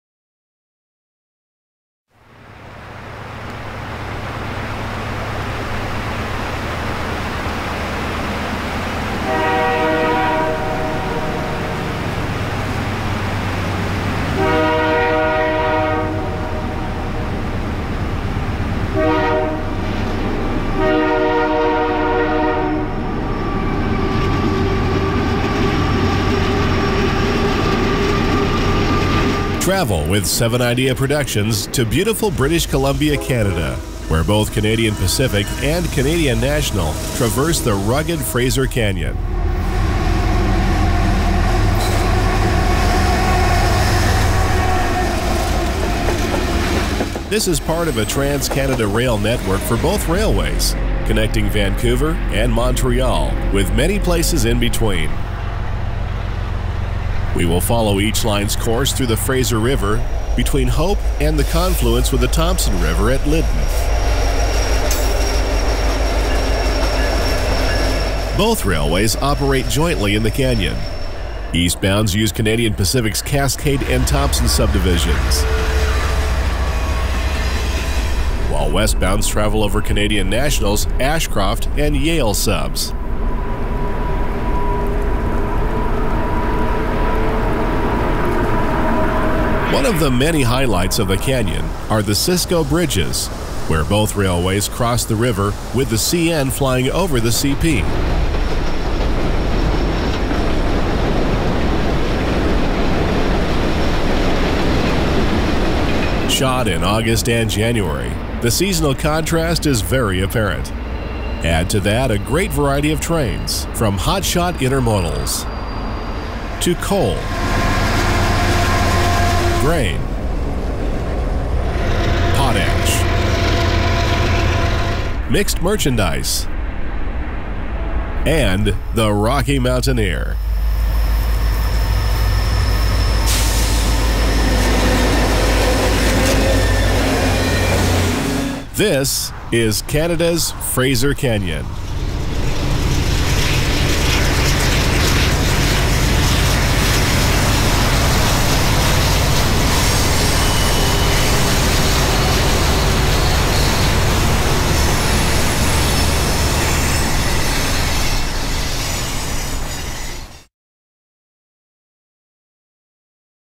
Canada's Fraser Canyon Preview
Travel with 7idea Productions to beautiful British Columbia, Canada where both Canadian Pacific and Canadian National traverse the rugged Fraser Canyon. Cutting a deep swath through the Cascade Range, this is an important link in the trans-Canada rail network connecting Vancouver with Montreal.   This program follows each line’s course along opposite banks of the Fraser River between Hope and the confluence with the Thompson River at Lytton. These competing railways operate jointly in the canyon. Eastbounds use Canadian Pacific’s Cascade and Thompson Subdivisions, while westbounds travel over Canadian National’s Ashcroft and Yale Subs. One of the many highlights of the canyon are the Cisco Bridges where both railways cross the river with the CN flying over the CP.   Our summer and winter visits reveal the grand seasonal contrast of the canyon’s appearance. Add to that a great variety of trains from hot shot intermodals, to coal, grain, potash, mixed merchandise, and the Rocky Mountaineer and you will know why railfans the world over love visiting Canada’s Fraser Canyon!  This program was shot between July 2016 and January 2017.

1 Hour 37 Minutes